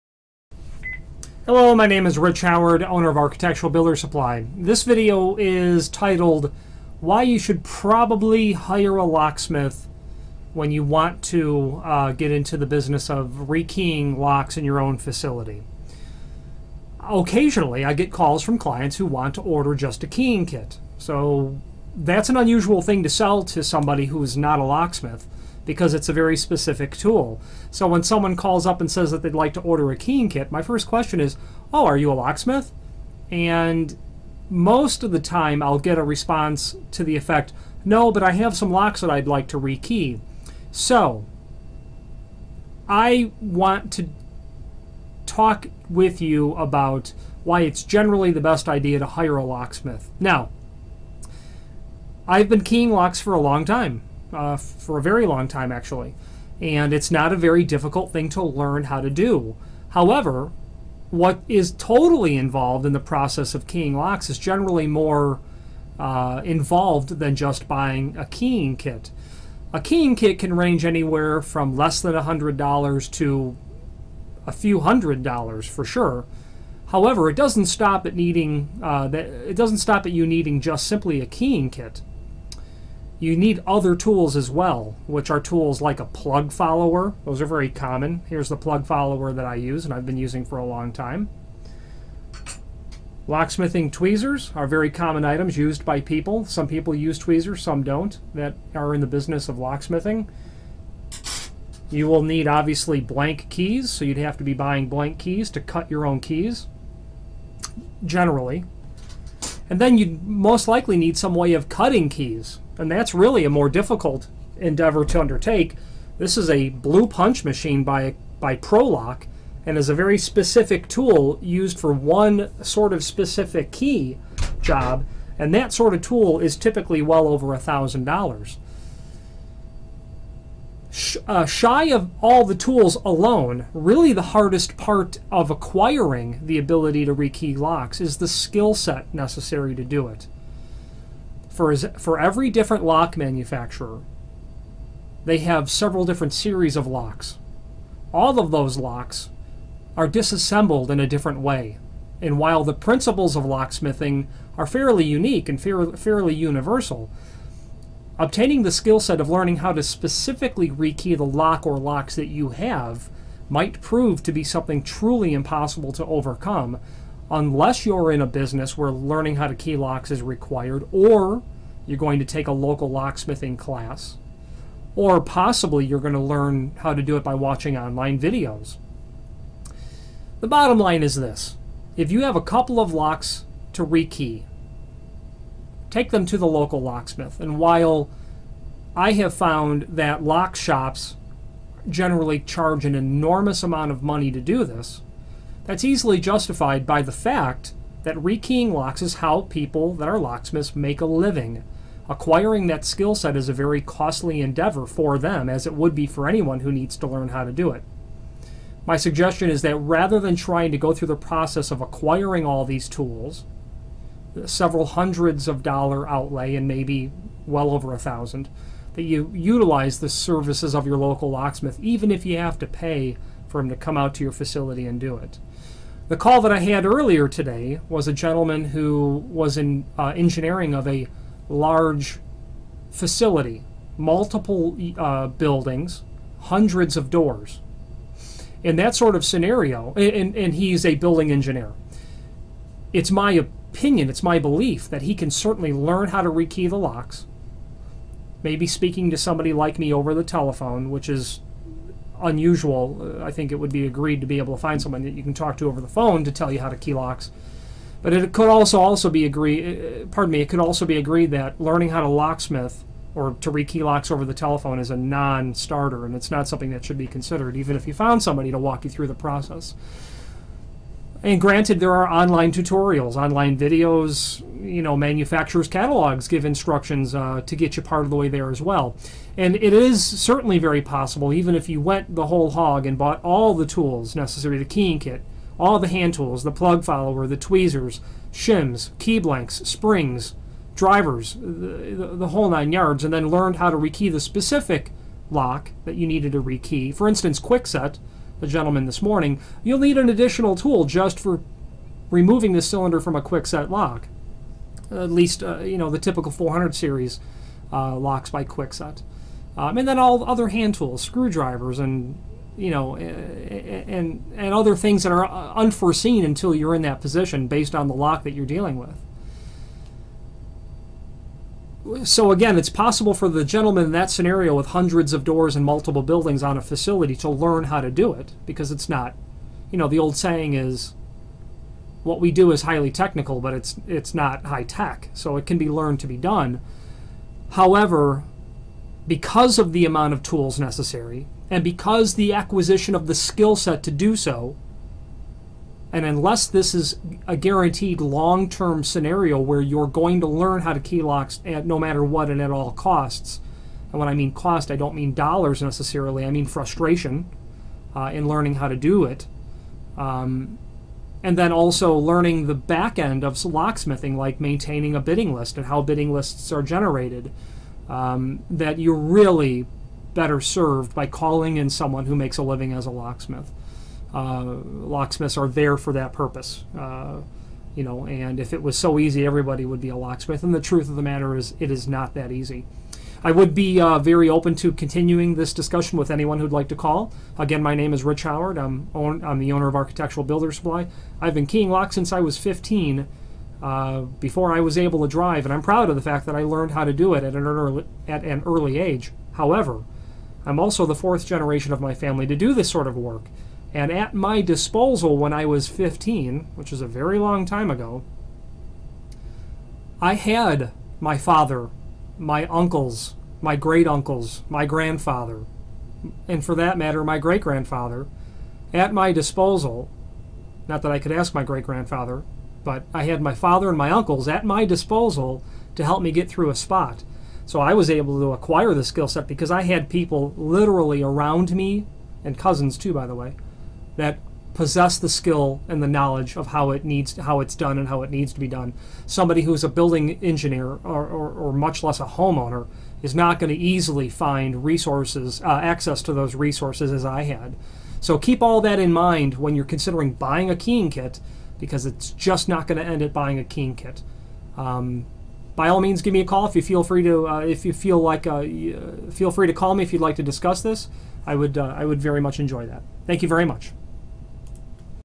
Why you should hire a Locksmith when you want to get into a business of re-keying locks
This video discusses why you should probably hire a Locksmith when you want to get into a business of re-keying locks in your own facility.
This video is titled "Why you should probably hiring a Locksmith when you want to get into a business of re keying locks in your own facility.
Occasionally i get calls from clients who wants to order just the keying kit, that's an unusual thing to sell to somebody that is not a locksmith because is a very specific tool.
I want to talk with you about why is generally best idea to hire a locksmith.
I have keying locks for a very long time and it's not a very difficult thing to learn how to do, however what is totally involved int the process of keying locks is generally more involved of just buying a key kit.
It doesn't stop of you need just a keying kit, you will net the all tools.
-plug follower
-lock smith tweezers,  very common used by people
-blank keys
You need a machine for cutting keys.
You also need a skill set to do it .
For every different lock manufacturer they have several different series of locks. All of these locks are dissembled in different ways.
The bottom line is this: if you have a couple of locks to re key take them to the local lock smith place.
My suggestion is that rather than of going to the process of acquiring all these expensive tools, several hundred dollars or maybe more of a thousand.
Taking phone instructions is not indicated should not be consider even if you find somebody to walk you through the proses, manufacturer instructions as well as video tutorials etc as you still need the tools.
It could be learn how to be done however because of the amount tools necessary and because of the position of the skills to do so you are really better of calling a lock smith professional.
It is not easy to install as well.
I will be open to continue this discussion with anyone.